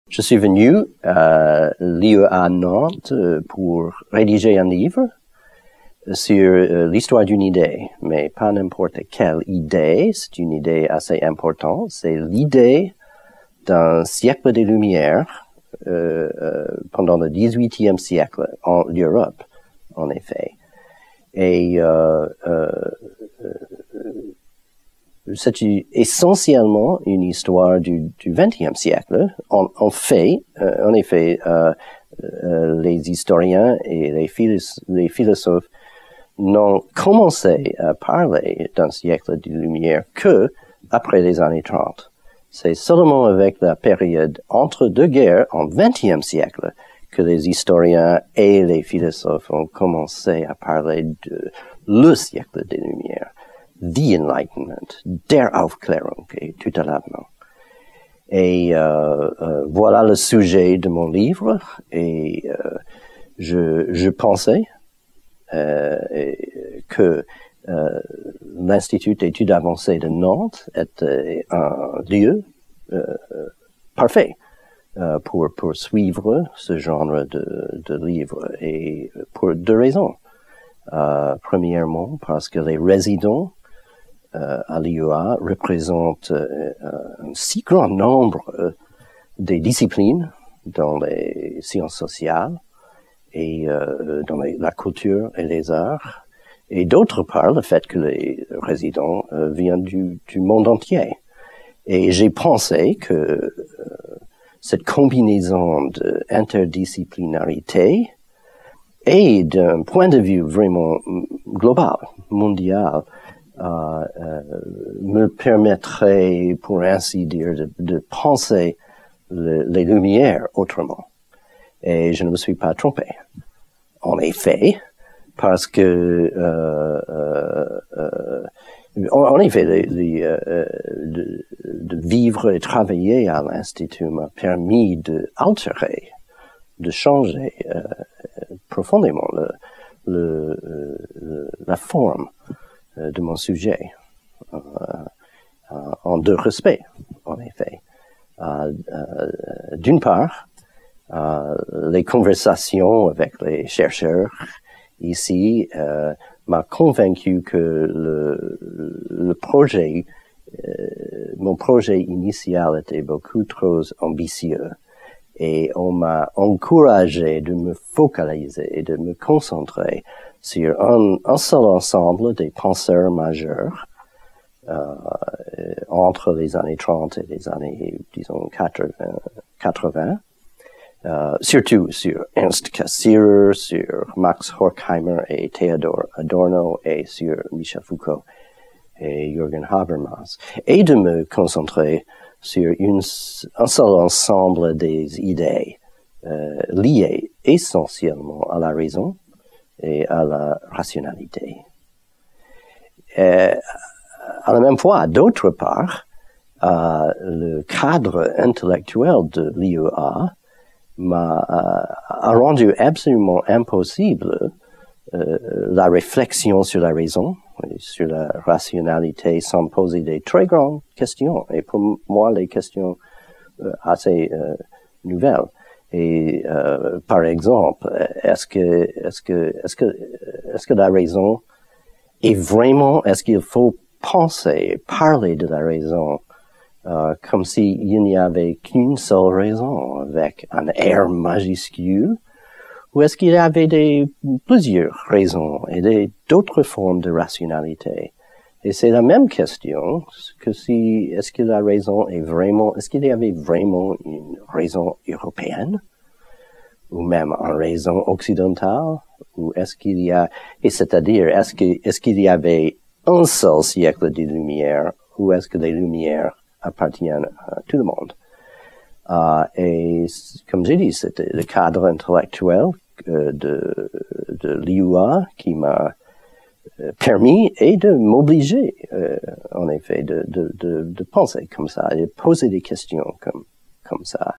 Johnson Kent Wright - « Miroir clair et lumineux » : le siècle des Lumières dans la pensée moderne
C’est Ernst Cassirer qui, en 1932, a qualifié les Lumières de « miroir clair et lumineux » dans lequel les Européens, à un moment de crise, pouvaient entrevoir les meilleurs aspects de leur identité. Aujourd’hui, alors que les Lumières sont redevenues une référence centrale dans les débats sur la modernité au niveau mondial, il est plus urgent que jamais de revenir sur les usages – et les abus – de cette idée. Le but de Johnson Kent Wright est de compléter un livre, qui considérera l’histoire de la réflexion sur le siècle des Lumières, comme un objet à la fois d’érudition et de polémiques philosophique et politique, au cours du siècle dernier. Son argument principal sera livré sous la forme d’une périodisation, en passant des grandes défenses antifascistes des Lumières à partir de l’entre-deux-guerres aux nouvelles attaques du marxisme occidental et du post-structuralisme français dans l’après-guerre, puis du calme relatif de « l’industrie des Lumières » grandissante dans les dernières années du siècle à la conjoncture actuelle, au moment où l’identification passionnée avec les Lumières, pour de multiples raisons, est redevenue à l’ordre du jour.

Biographie

Johnson Kent Wright est professeur à la faculté « Pensée civique et économique » de l’Arizona State University, aux États-Unis. Par le passé, il a, dans cette même université, enseigné au département des humanités interdisciplinaires et au département d’histoire. Né dans l’État du Michigan, il a obtenu son doctorat en histoire de l’université de Chicago en 1990. Son travail s’est concentré sur trois domaines : 1) la pensée politique française à l’époque des Lumières – Mably, Montesquieu, et Rousseau en particulier ; 2) l’histoire politique des « révolutions atlantiques », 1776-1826 ; et 3) l’historiographie des Lumières. Au cours de sa carrière, il a occupé les fonctions d’éditeur de la revue French Historical Studies.